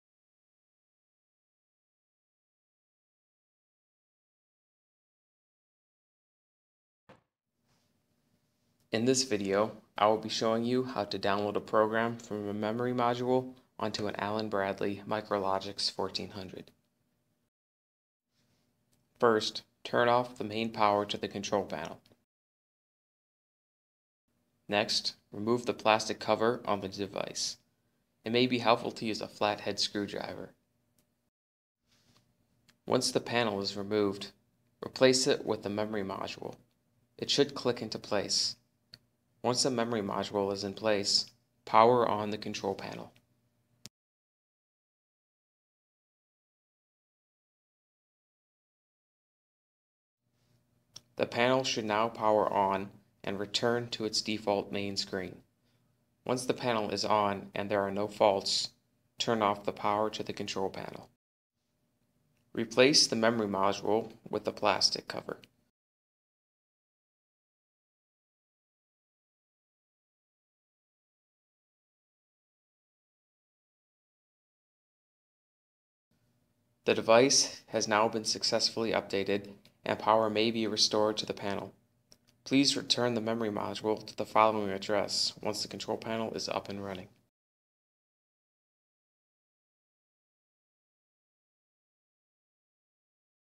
Download a Program from Memory Module to MicroLogix 1400 PLC
This video will briefly show you how to download and load a program from a memory module on to a MicroLogix 1400 Programmable Logic Controller (PLC).